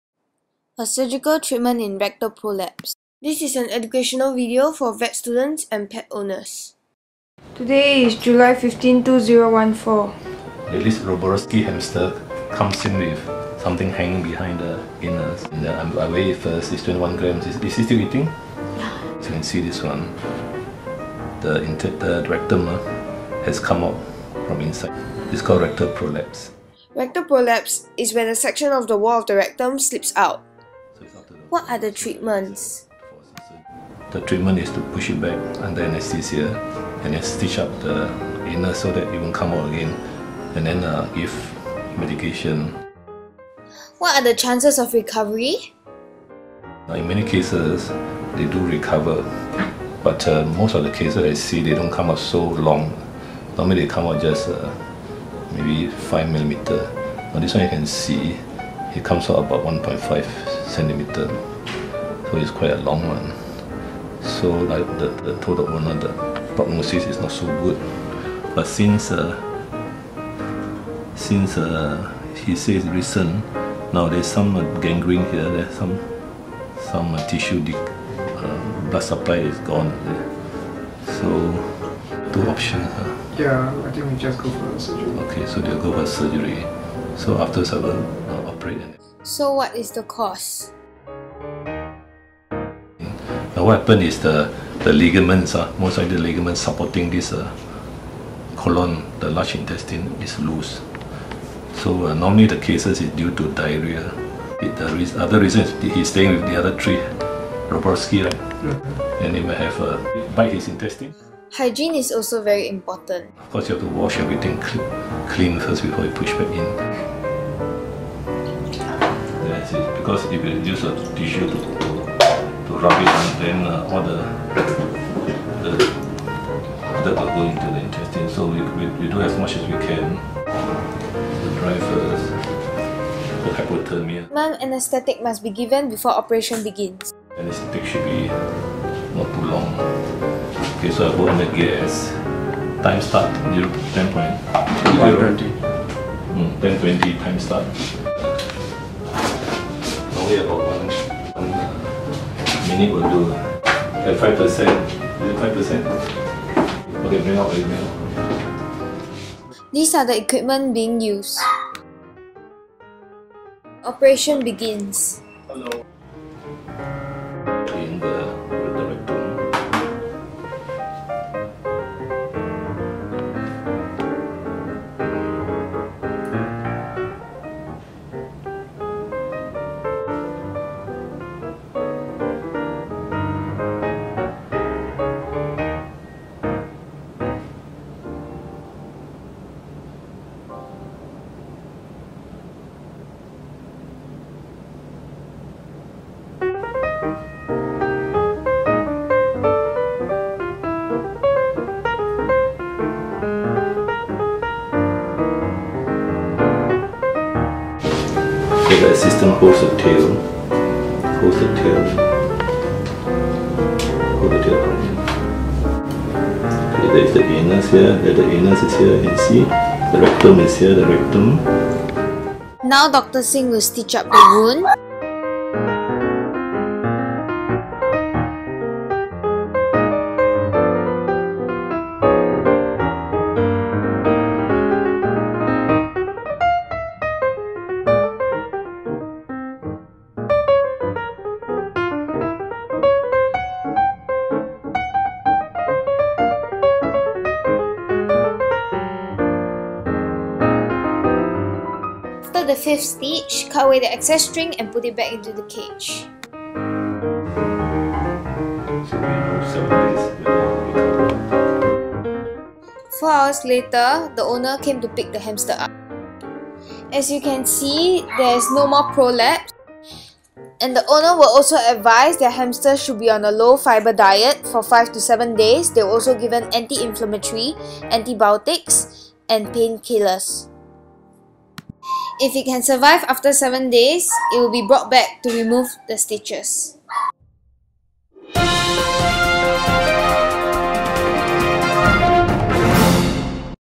Rectal Prolapse in a Roborovski hamster
Rectal prolapse is uncommon in dwarf hamsters. The cause is unknown in most cases. The prognosis is poor if the rectal prolapse is extensive as in this case. The purse string suture closes the anus.

Generally, with medication, the rectum goes back to normal size inside the rectum in most dogs and cats. For this hamster, she survived the anaesthesia.  .

Video is produced by an intern and has not been edited by Dr Sing.